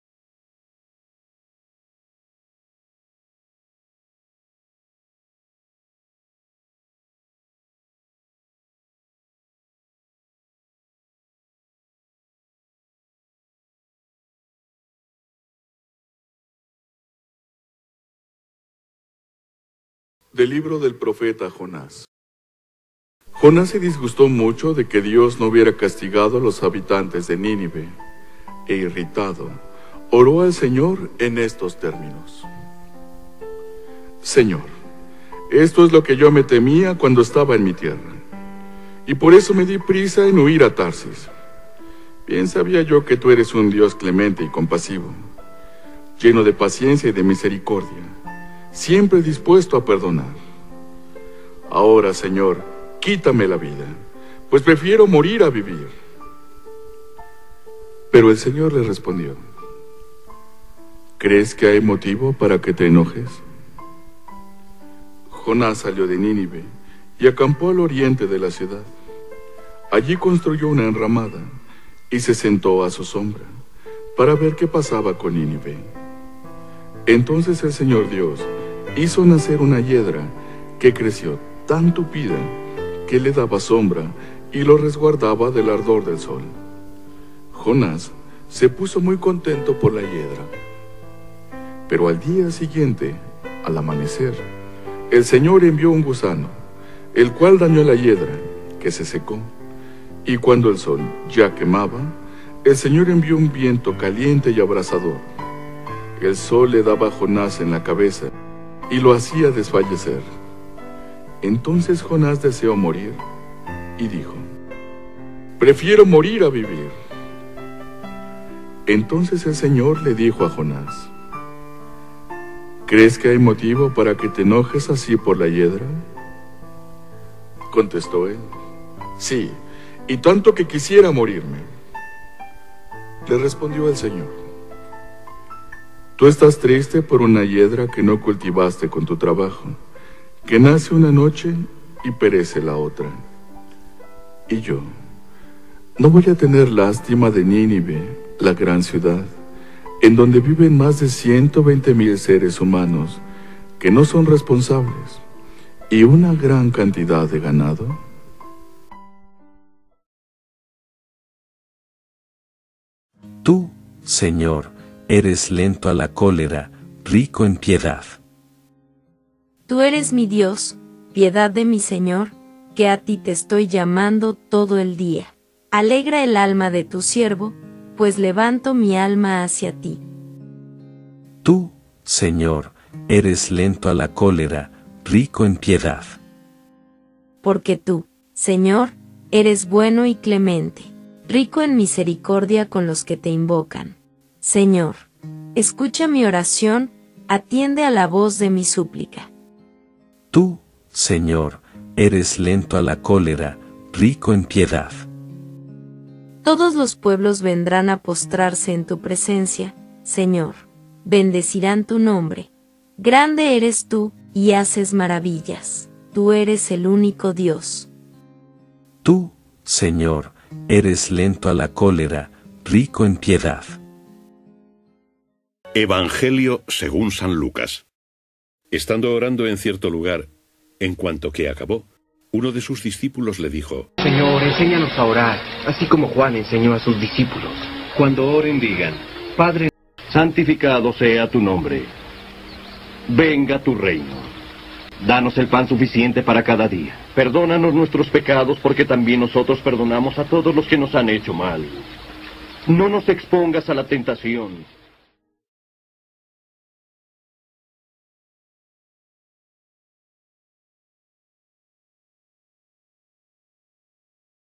MIÉRCOLES OCTUBRE 6 DE 2021 | LUCAS 11,1-4
LECTURAS Y EVANGELIO EUCARISTÍA MIÉRCOLES OCTUBRE 6 DE 2021 MIÉRCOLES XXVII SEMANA DEL TIEMPO ORDINARIO CICLO B
Primera Lectura: la profecía de Jonás 4,1-11 Salmo 85,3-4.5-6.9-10 R/. Tú, Señor, eres lento a la cólera, rico en piedad Evangelio Lucas 11,1-4